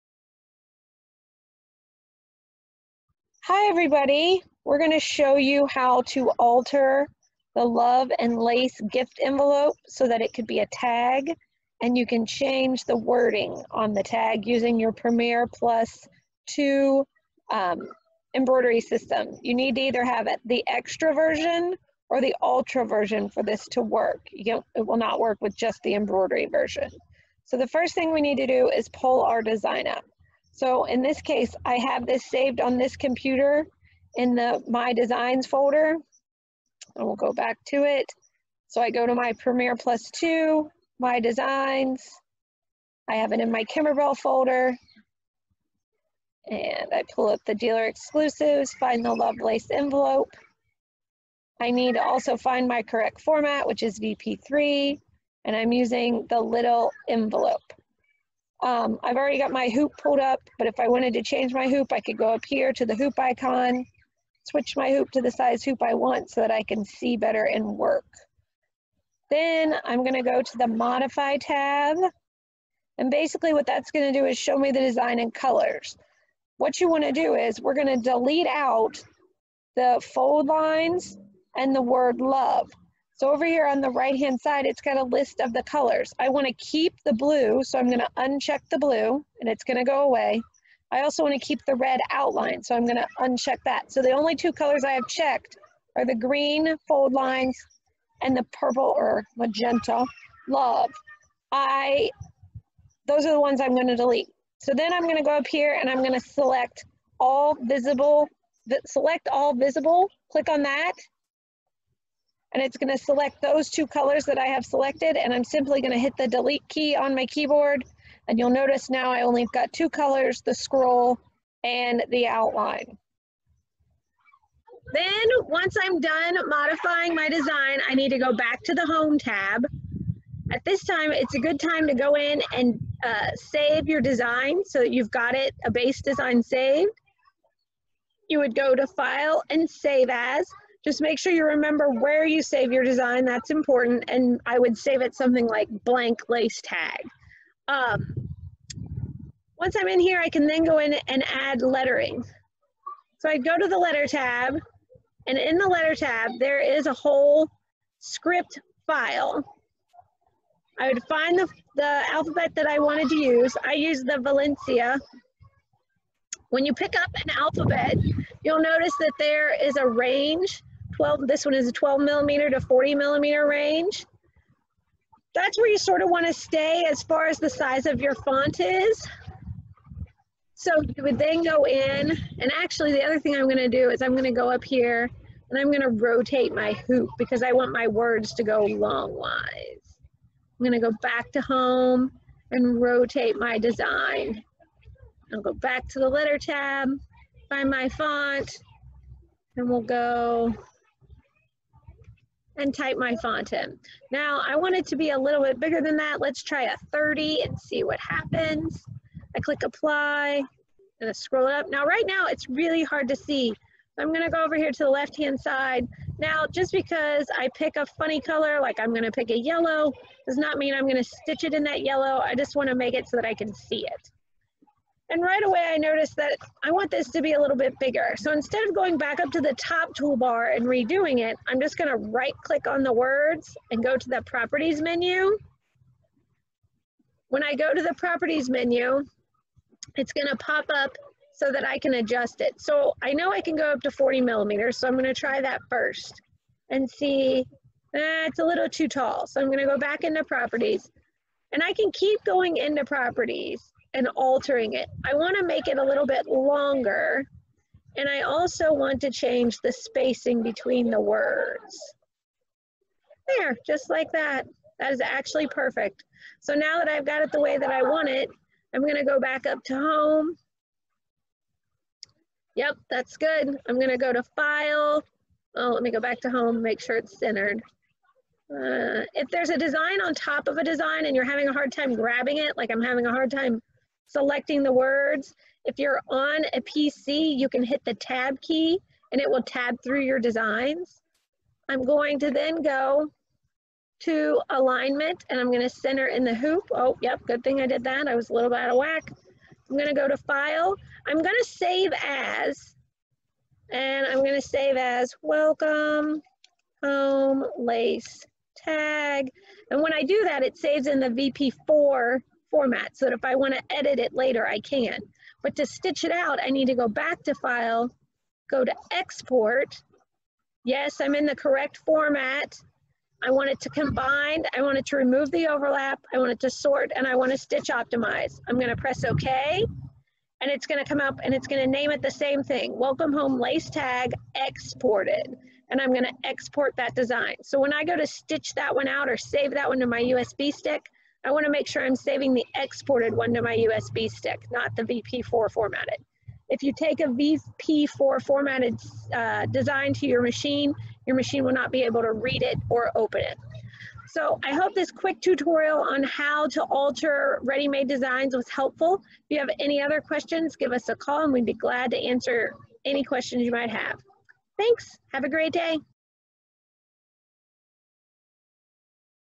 Editing in the Modify Tab in Premier+2 Embroidery Software
Learn how to edit embroidery designs in the Modify Tab in the Premier+2 Embroidery Software. We will show you how to delete stitching in an already digitized embroidery design and add your own stitching. This works in the extra or ultra version.